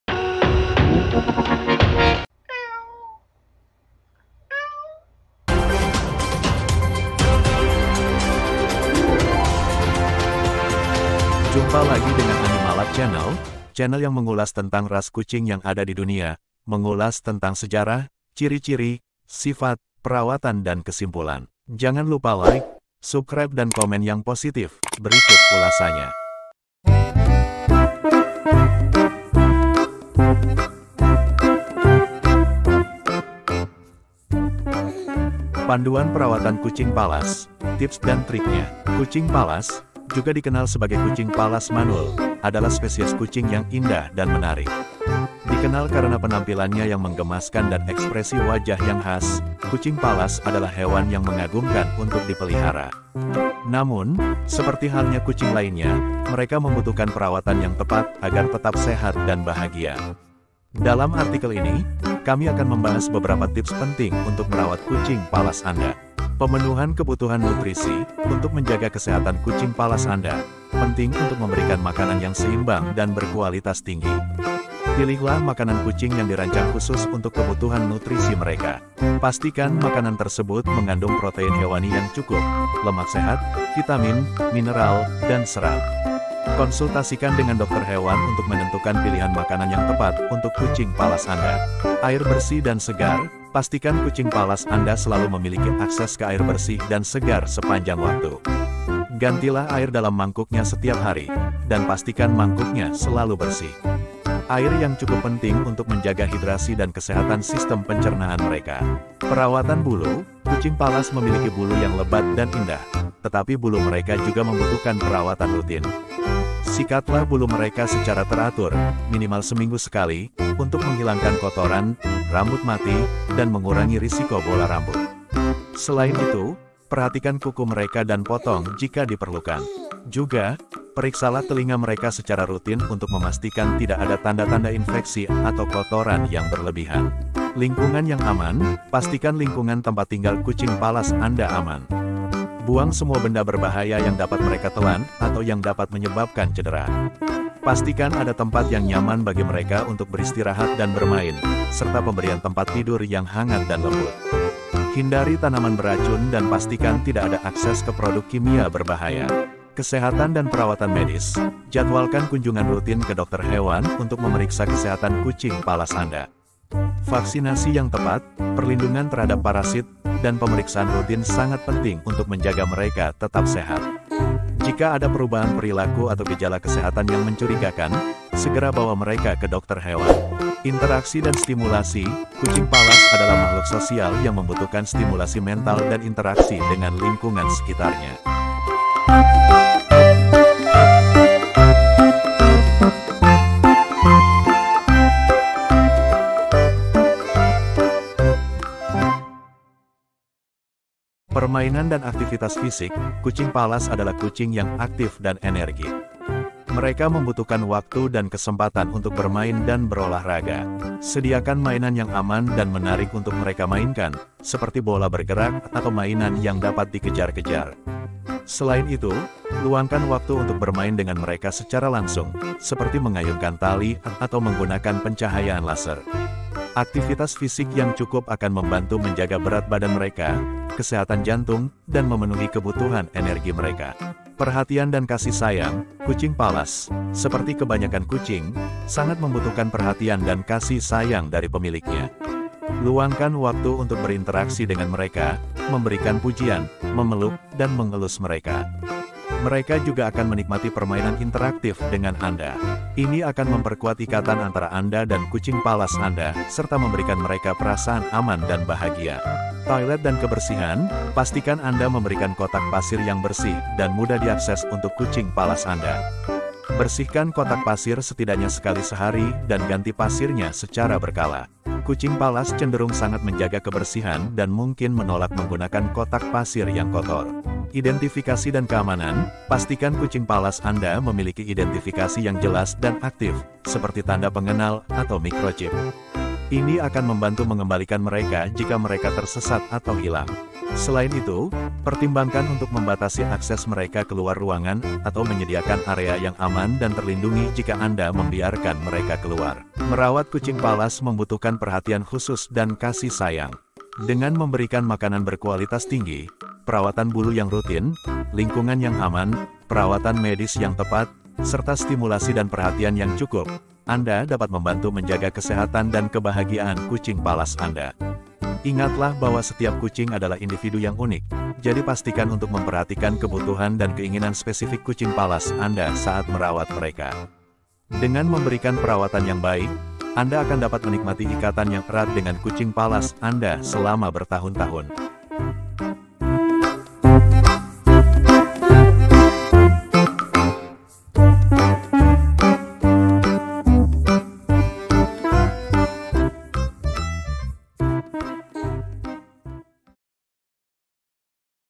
Panduan Perawatan Kucing Pallas Tips Dan Trick#Kucing
Vidio ini menjelaskan tentang Sejarah hewan,karakteristik hewan,cara Perawatan hewan,yang baik dan benar,semoga bermanfaat..
 Panduan Perawatan Kucing Pallas,Tips dan Trik#kucing

Kucing Pallas, juga dikenal sebagai kucing Pallas Manul, adalah spesies kucing yang indah dan menarik. Dikenal karena penampilannya yang menggemaskan dan ekspresi wajah yang khas, kucing Pallas adalah hewan yang mengagumkan untuk dipelihara. Namun, seperti halnya kucing lainnya, mereka membutuhkan perawatan yang tepat agar tetap sehat dan bahagia. Dalam artikel ini, kami akan membahas beberapa tips penting untuk merawat kucing Pallas Anda.
Merawat kucing Pallas membutuhkan perhatian khusus dan kasih sayang. Dengan memberikan makanan berkualitas tinggi, perawatan bulu yang rutin, lingkungan yang aman, perawatan medis yang tepat, serta stimulasi dan perhatian yang cukup, Anda dapat membantu menjaga kesehatan dan kebahagiaan kucing Pallas Anda. Ingatlah bahwa setiap kucing adalah individu yang unik, jadi pastikan untuk memperhatikan kebutuhan dan keinginan spesifik kucing Pallas Anda saat merawat mereka. Dengan memberikan perawatan yang baik, Anda akan dapat menikmati ikatan yang erat dengan kucing Pallas Anda selama bertahun-tahun.